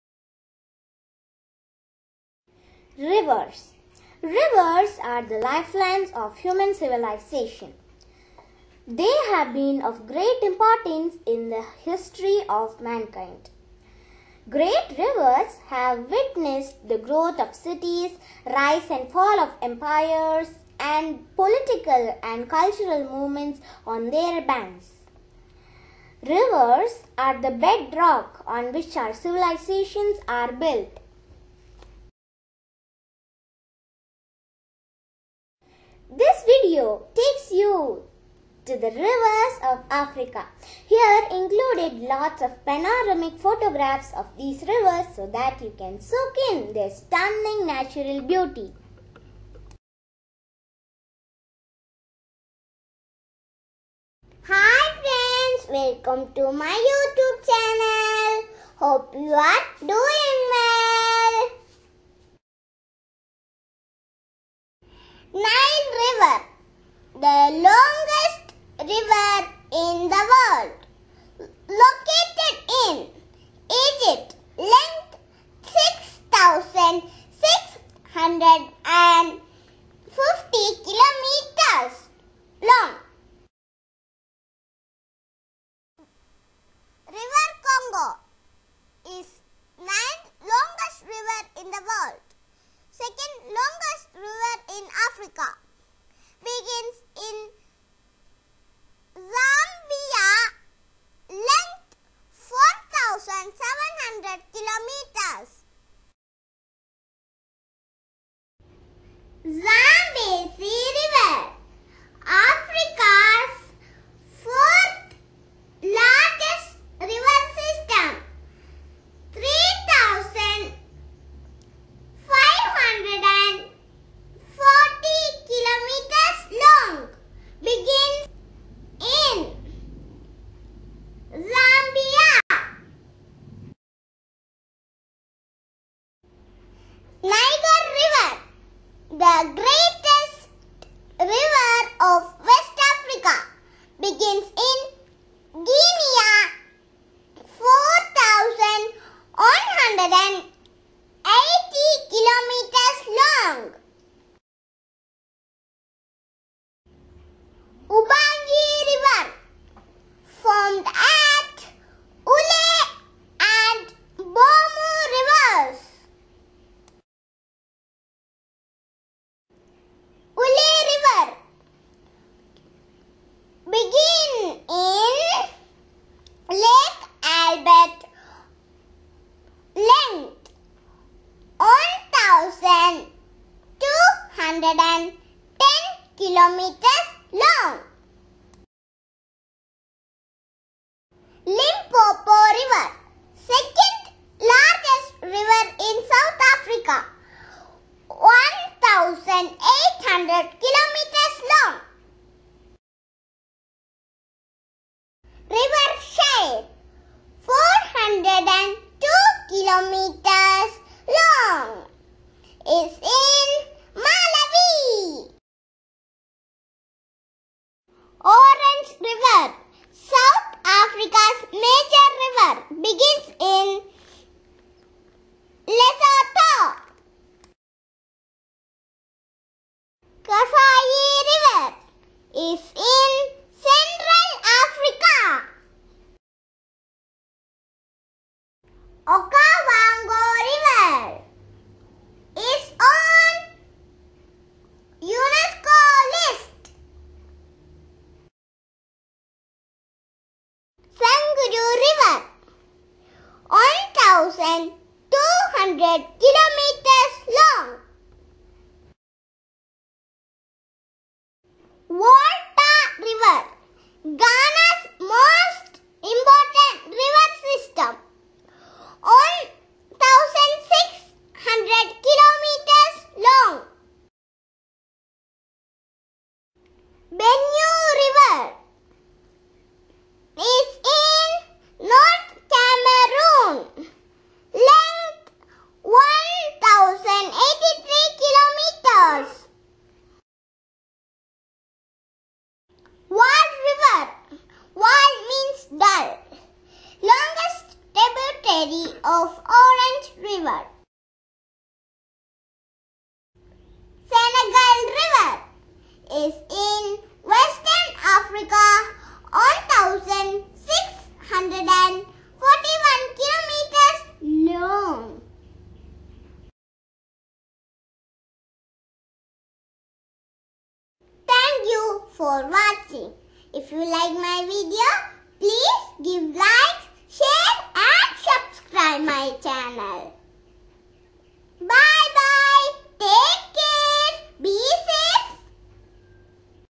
RIVERS
Rivers of Africa
Nile river
River Congo
Senegal River
Vaal River
Benue River
Volta River
Sankuru River
Okavango River
Kasai River
River Shire
Orange  River
Limpopo River
Uele River
Ubangi River
Niger River
Zambezi River